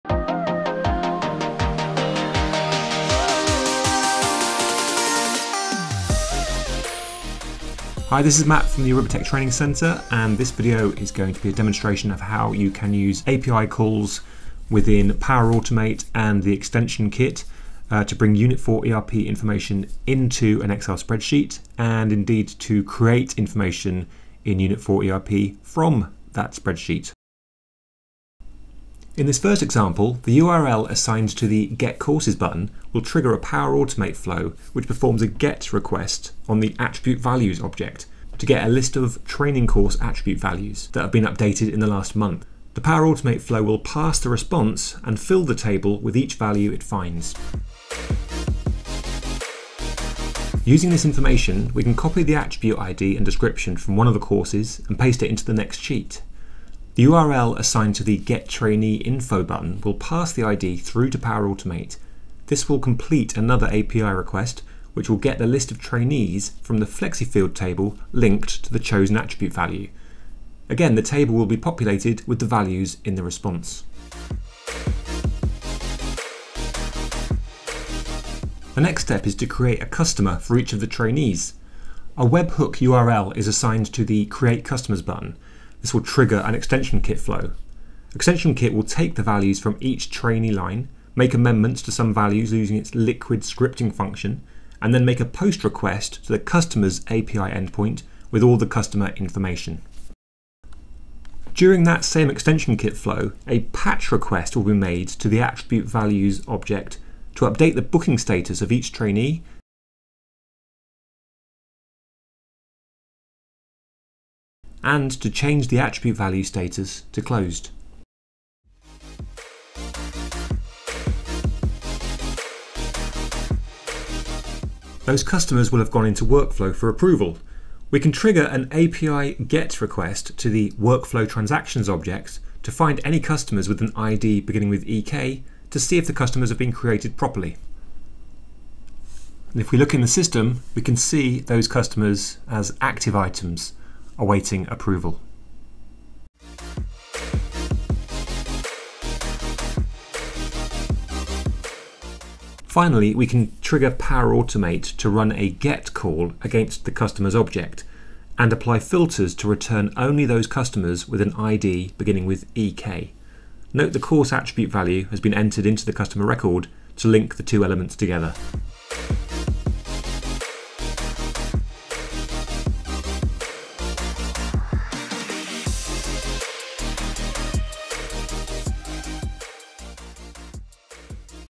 Using Power Automate and Extension Kit with Unit4 ERP.
I am really enjoying getting nerdy and creative with Web APIs and Extension Kit - two of the 1-day courses offered by the ATC - and thought I would test my skills!
I wanted to create a system where I can:

➡️ Get specific Attribute value info
➡️ Get values from a flexi-field table
➡️ Create Customers
➡️ Update Attribute values and flexi-field values
➡️ Check Customer workflow
➡️ Get new Customers info

...all without leaving my Excel spreadsheet. 🤯
This is a demo video where I show how I got on - enjoy!

If you would like to learn the skills needed to use Web APIs and Extension Kit, why not sign up to my courses! I hope to see you there. 😁